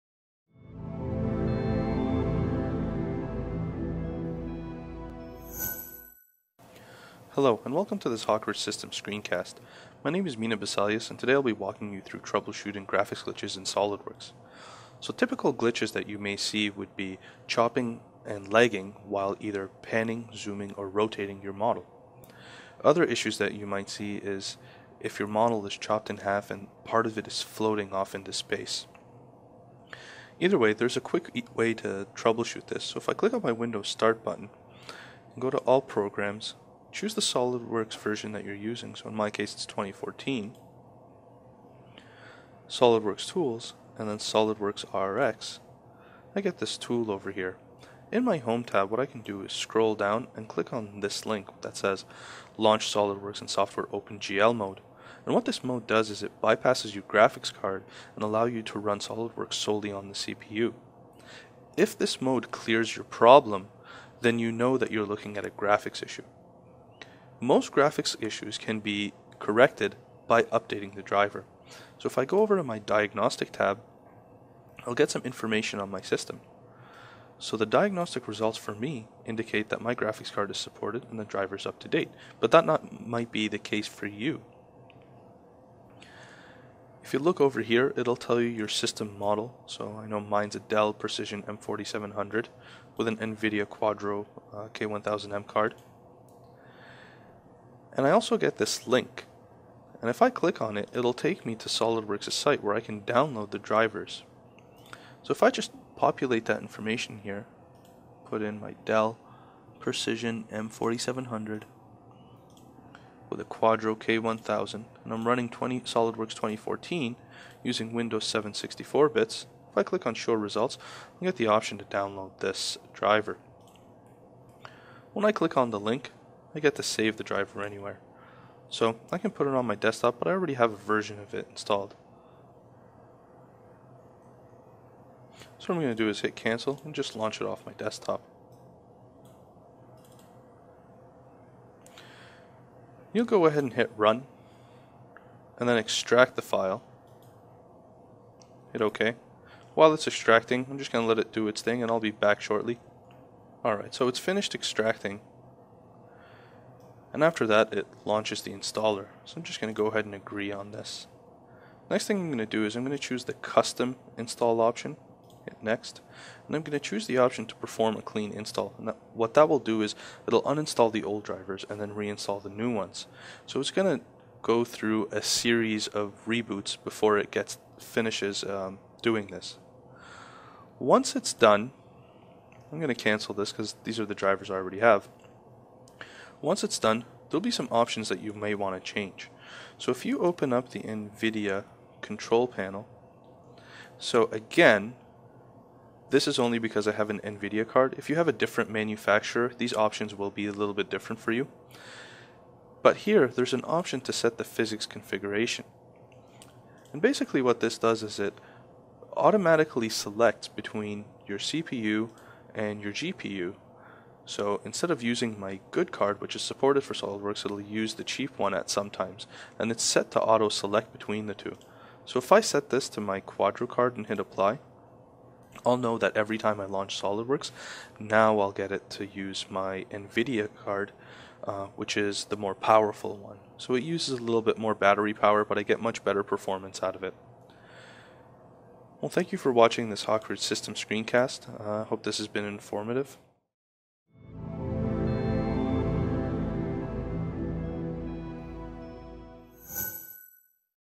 SOLIDWORKS - Troubleshooting Graphics Glitches
This video will show some common troubleshooting techniques used by tech support to diagnose common issues leading to graphics problems in SOLIDWORKS.